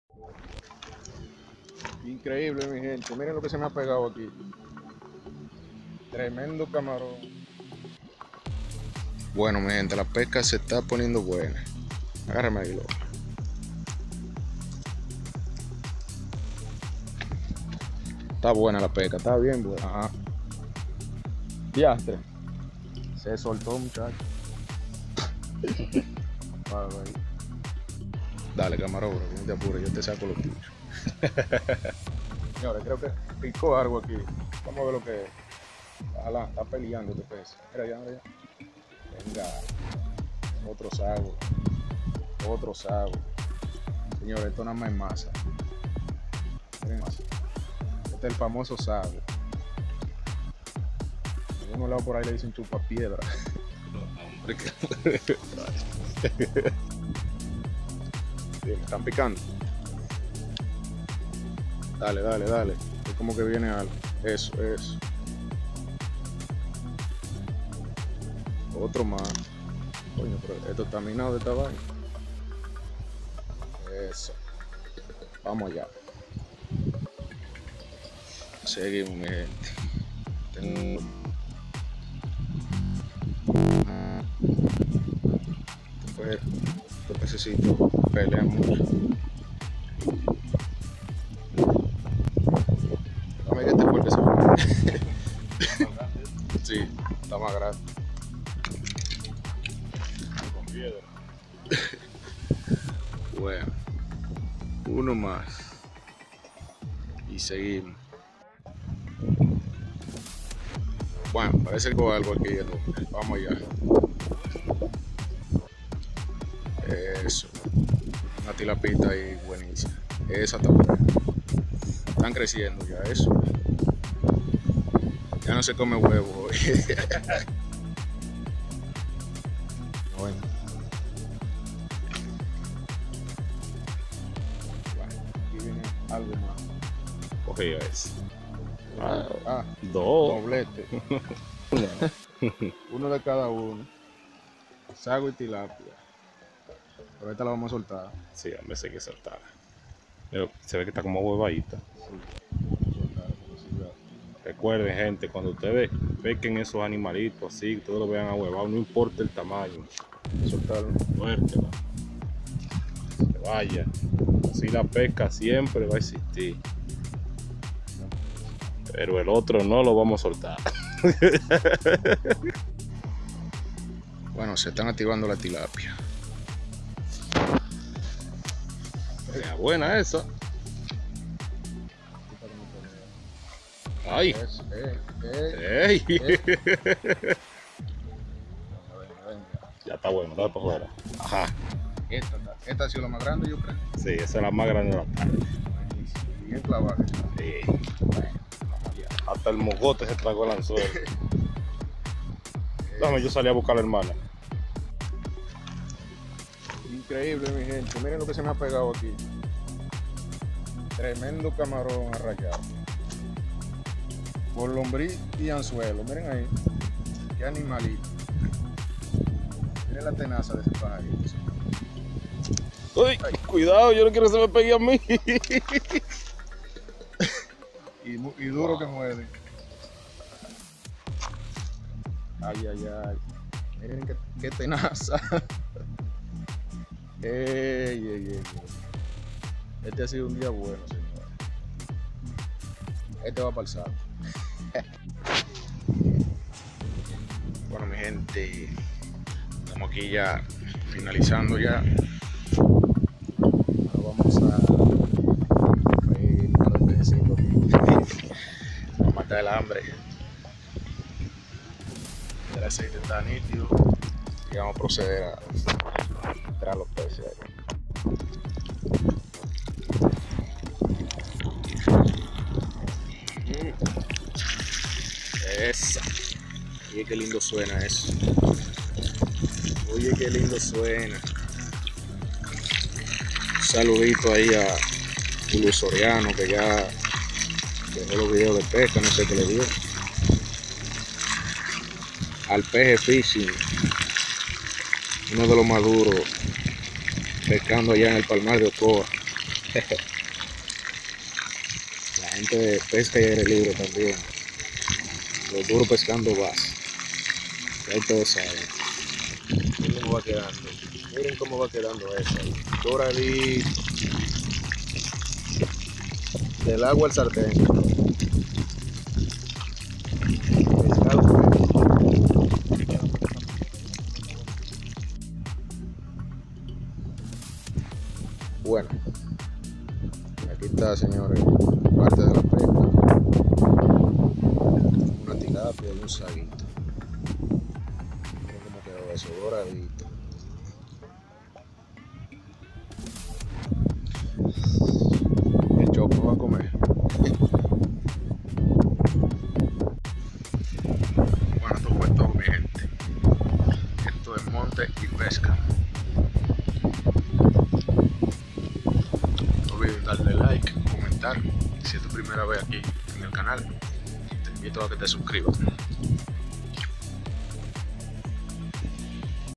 pesca y cocina tilapias ,sagos y camaron gigante |monte y pesca rd
pesca y cocina de tilapias ,sagos y camaron gigante de rio